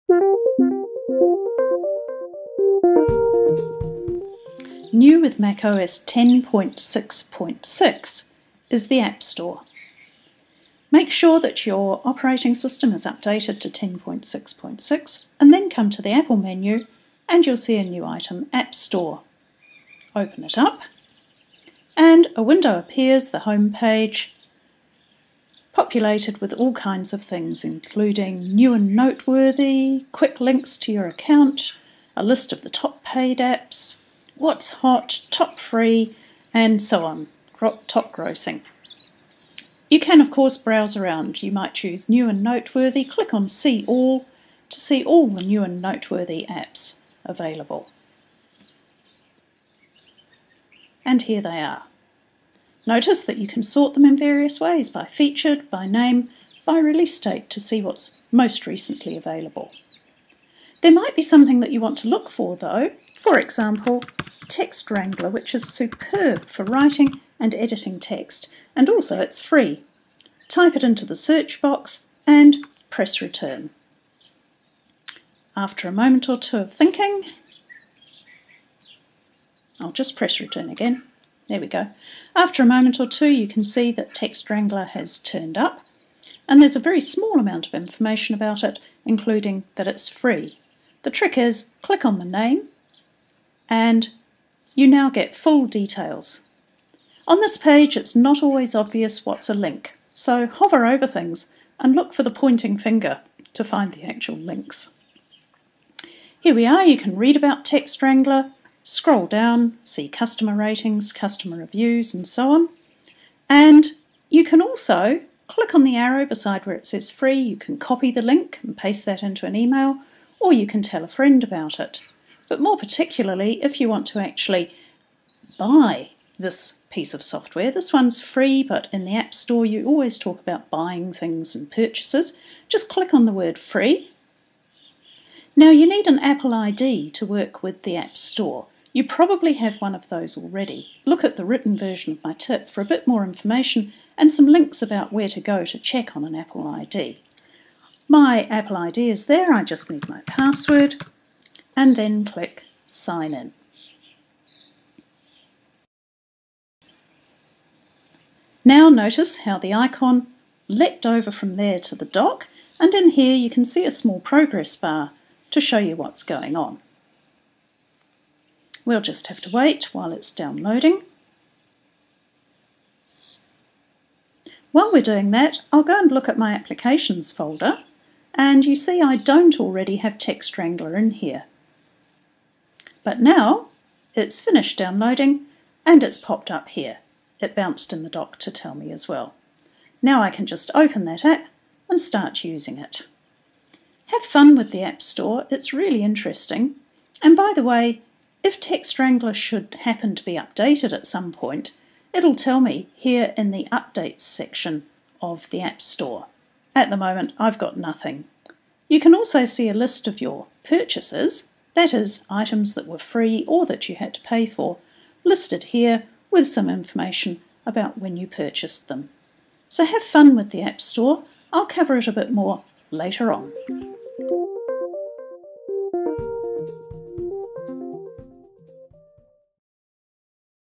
How to use the Mac App Store in Mac OS 10.6.6
The Mac App Store is new with Mac OS X 10.6.6. If you're already familiar with the App Store on iPhone or iPad then you'll easily find your way around. Otherwise it may be a bit confusing. This Tip starts you off on the right foot.

Read this and other Tips for users of Apple products